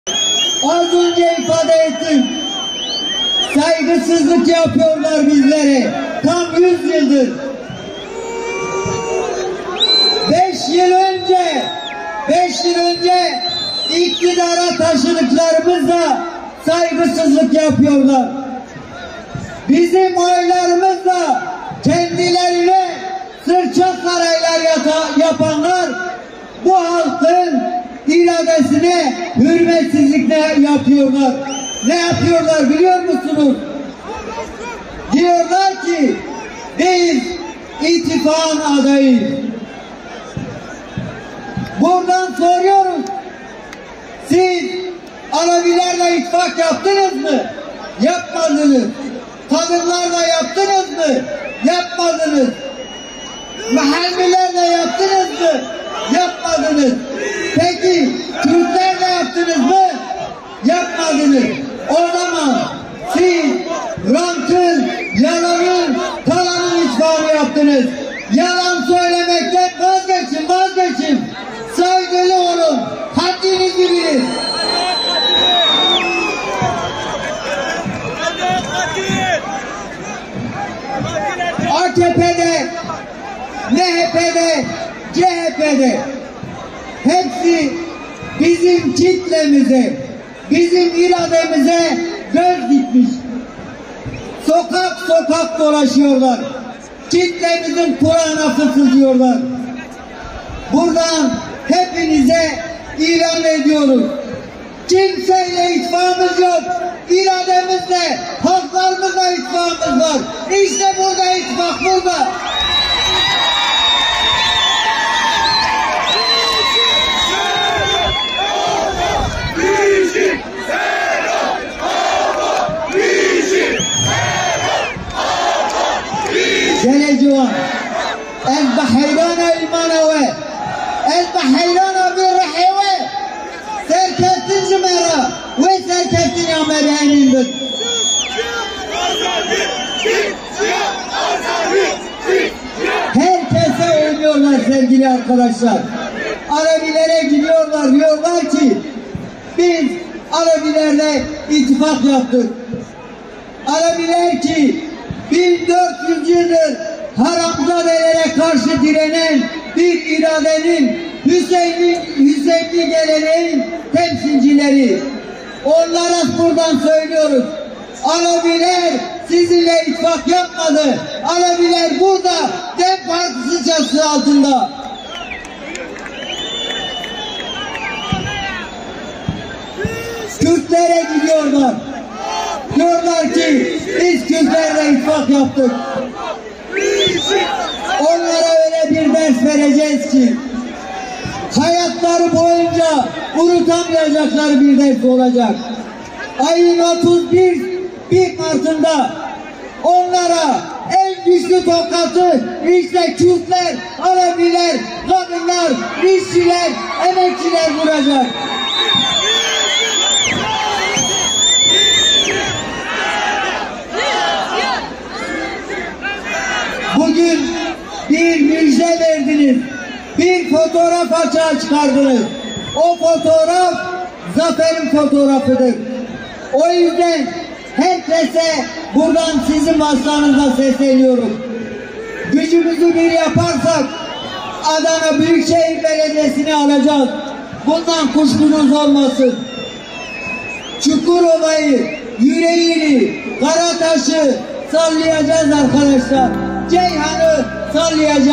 ADANA’DA CHP'YE ÇAĞRI: DEDİKODULARDAN VAZGEÇİN
Yerel seçimlere dair kentte CHP ile ittifak kurdukları yönündeki kimi söylemleri yalanlayan DEM Parti adayları, "Bu dedikodulardan bir an önce vazgeçin” çağrısı yaptı